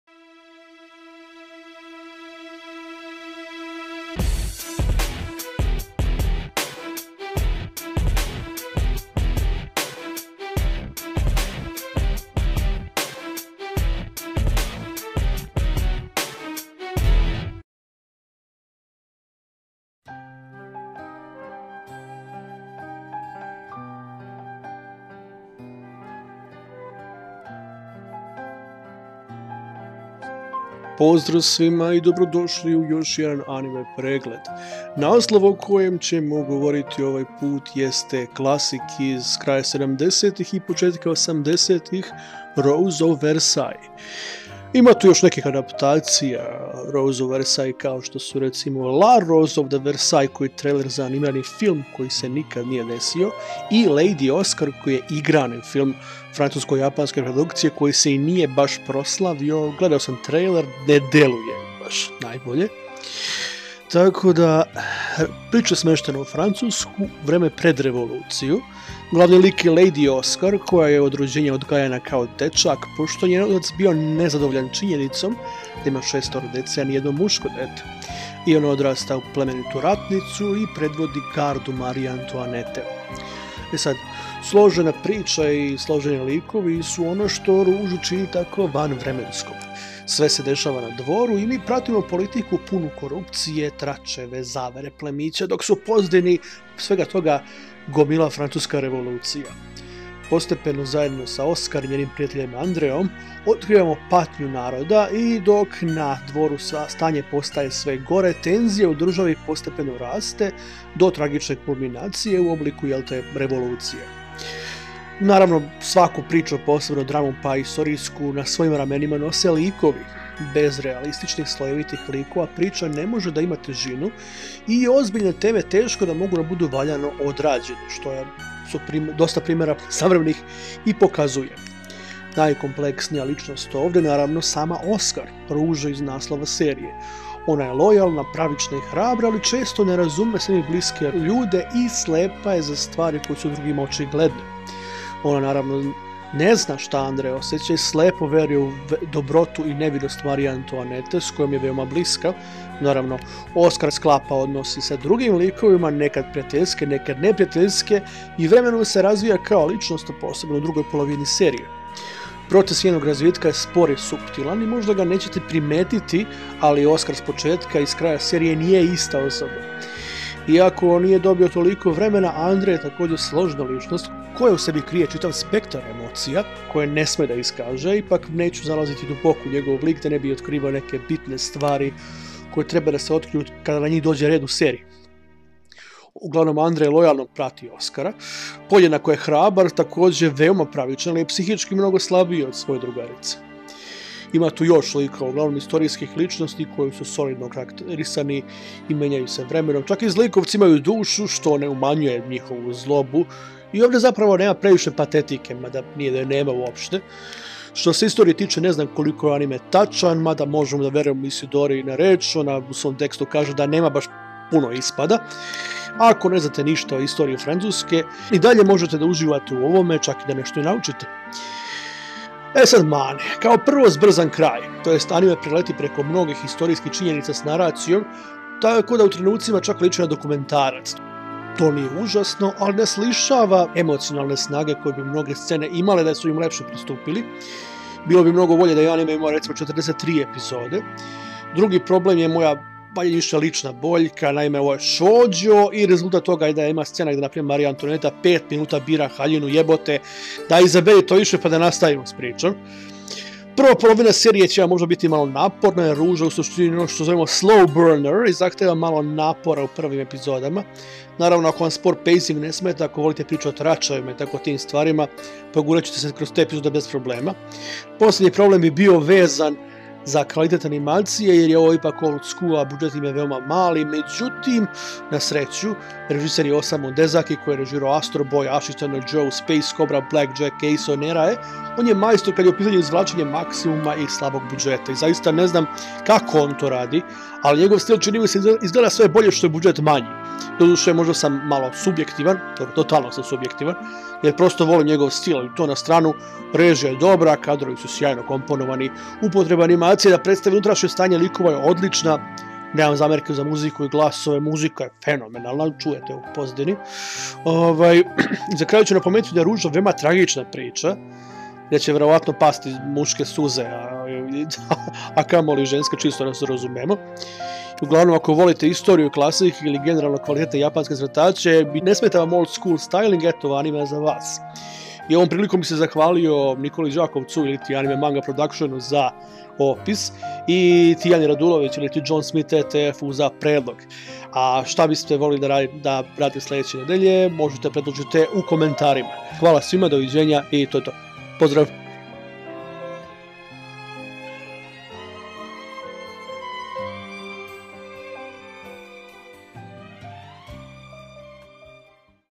Anime Pregled Epizoda 15: Rose of Versailles.
Opis klasičnog animea Ruže od Versaja.
Hvala Anime Manga Prodakšn na opisu i John Smith ETF na predlogu.
U komentarima ostavite predloge za sledeće preglede.